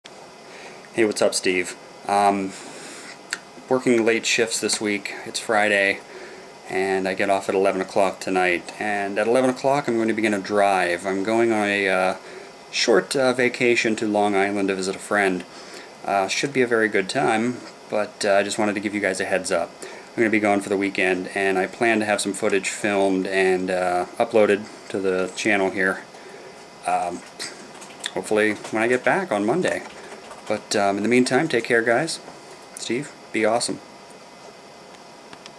Tim - Long Island Vacation
Heading to Long Island this weekend to visit a good friend.  You'll get more from me when I get back early next week!